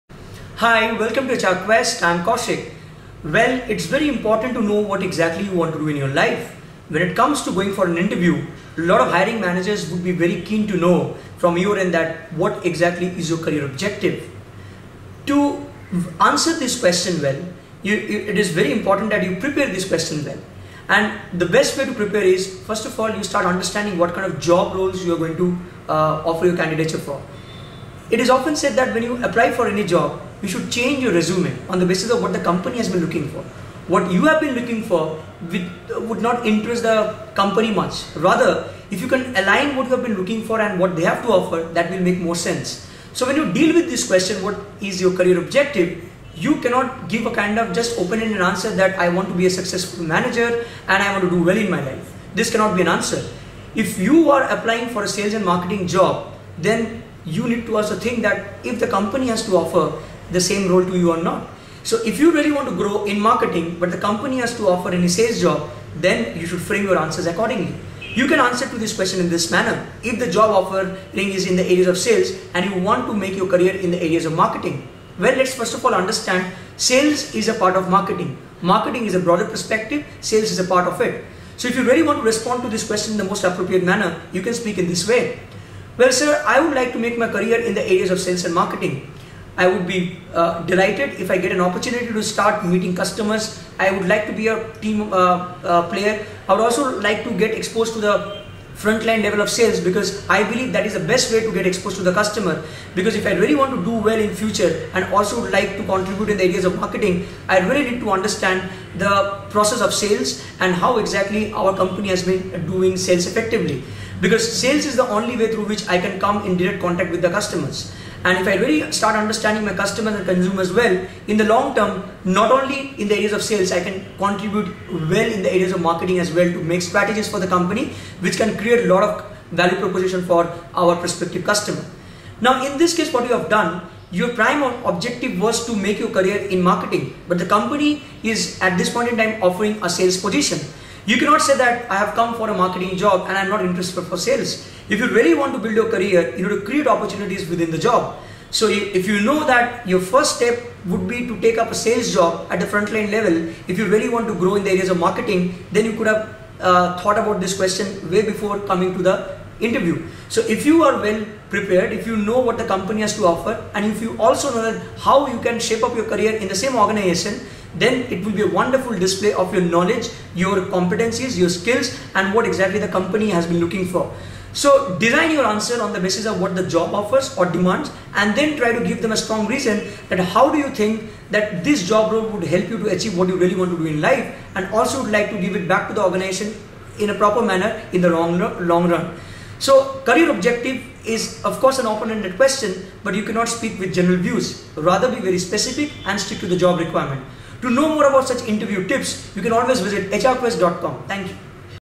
Frequently Asked Interview Questions - What Are Your Career Objective | HR Crest
Frequently Asked Interview Questions - What Are Your Career Objective
This is quite an open-ended question. And this questions needs to be handled carefully. The way you change your resume while applying for jobs, likewise you career objective also needs to be aligned with the job you are offering your candidature for.